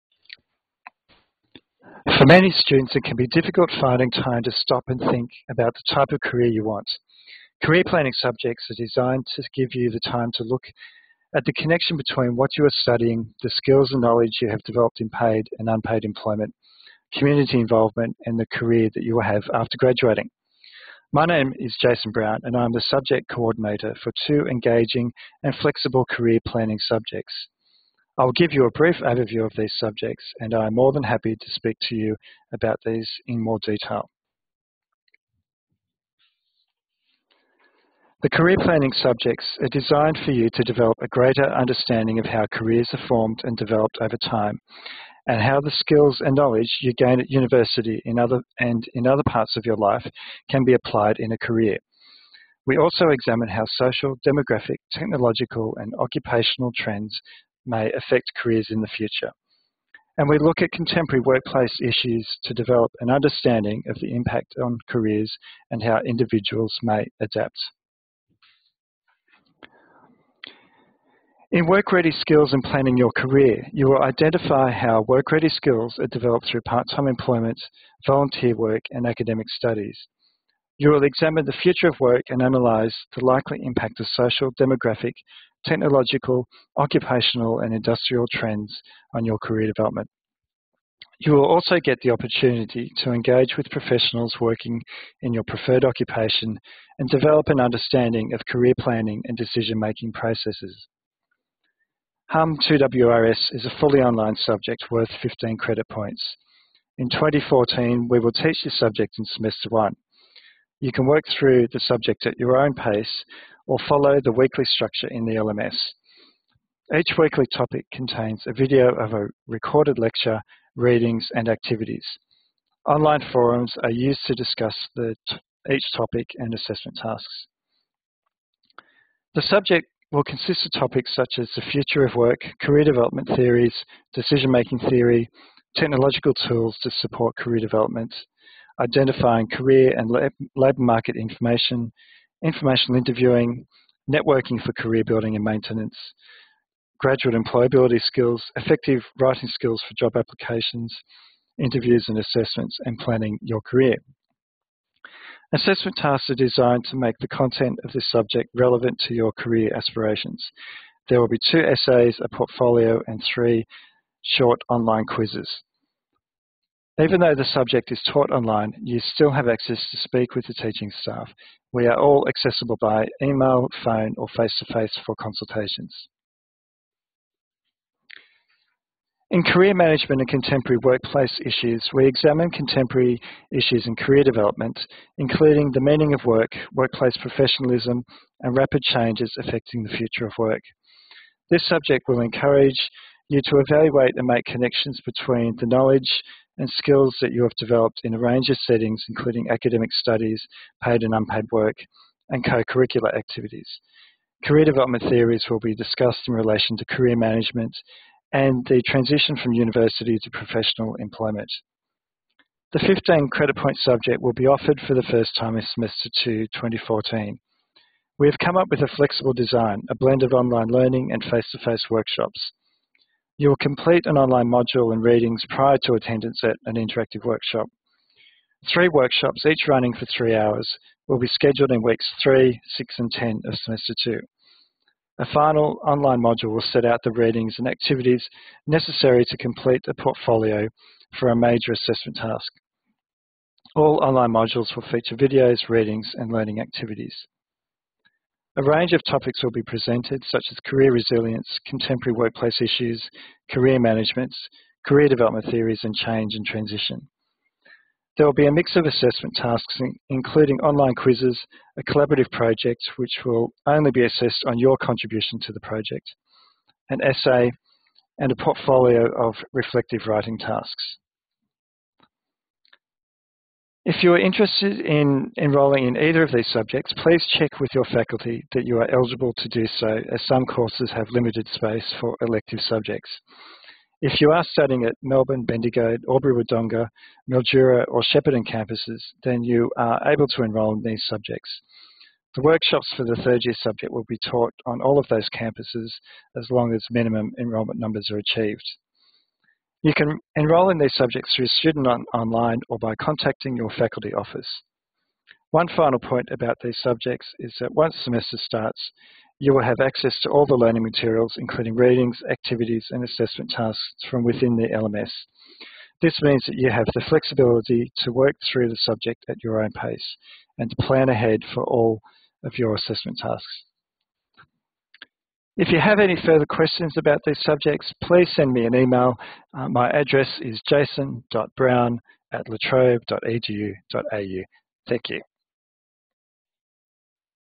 Career Planning Subjects 2014
La Trobe University offers two career planning subjects for undergraduate students. This presentation outlines each subject.